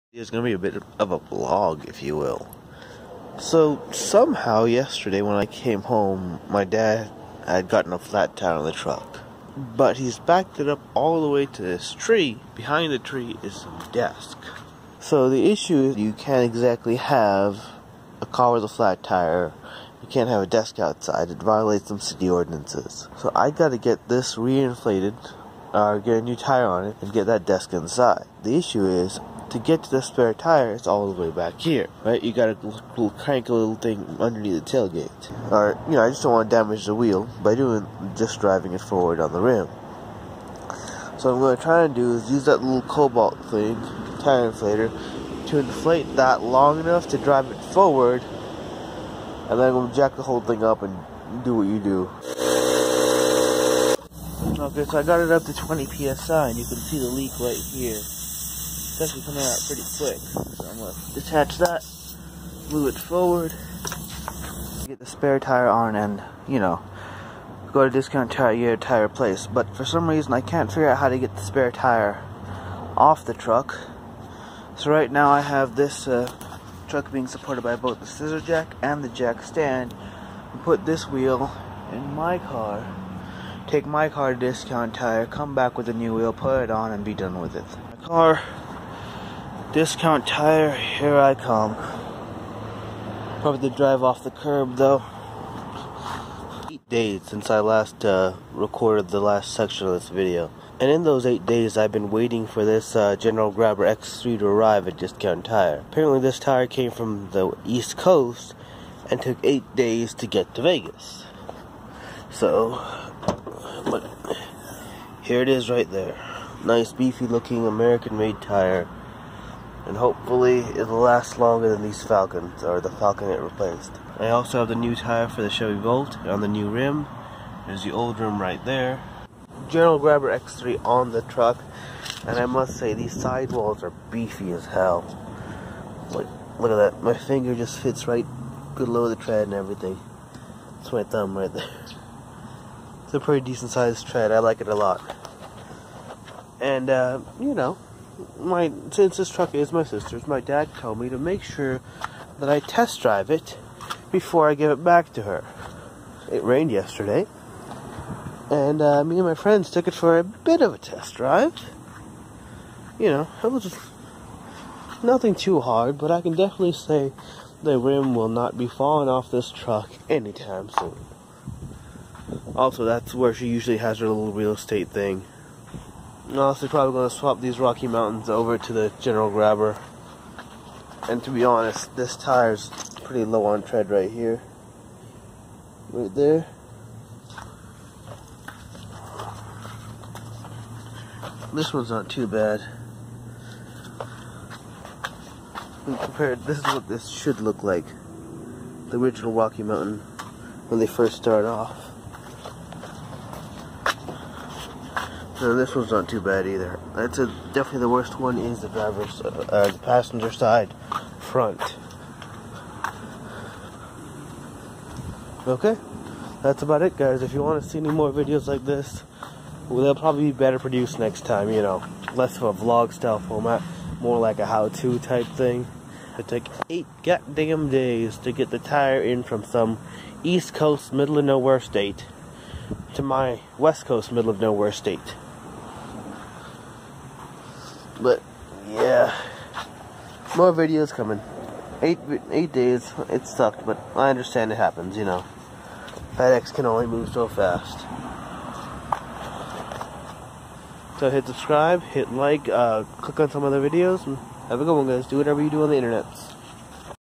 It Took Me 8 Days To Get A New Tire
Yes I got the desk inside to.